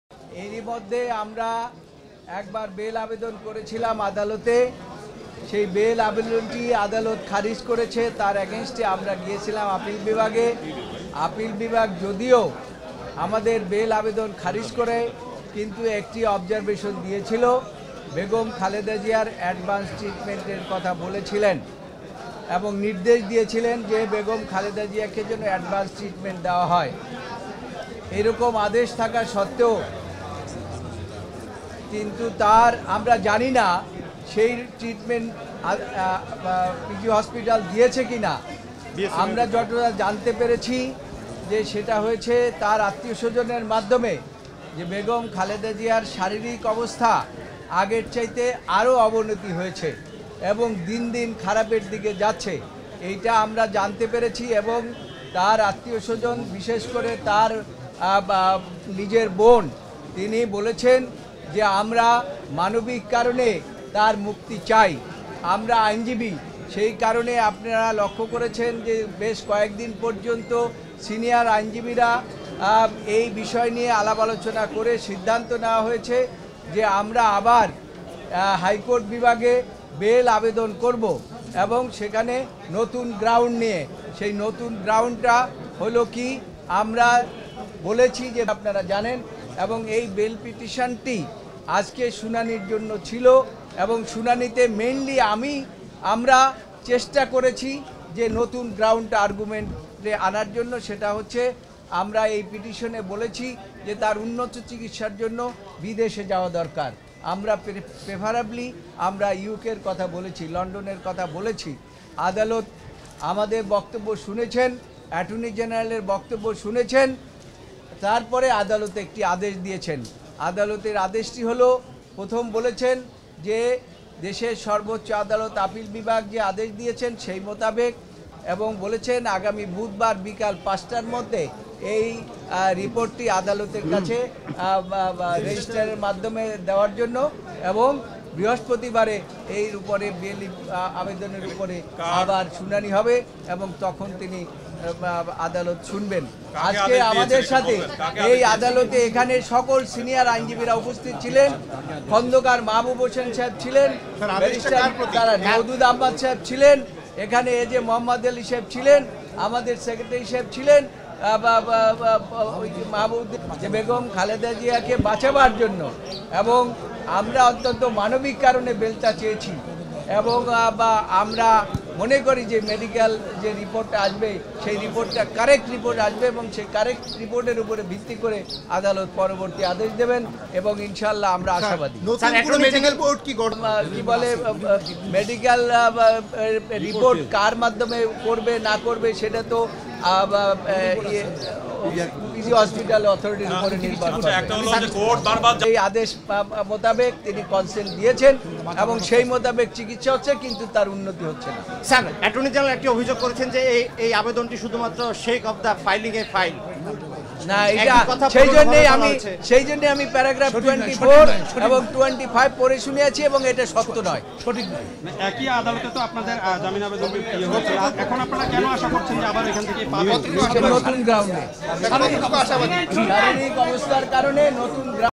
খালেদা জিয়ার জামিনের সবশেষ আপডেট জানালেন আইনজীবী
khaleda ZIA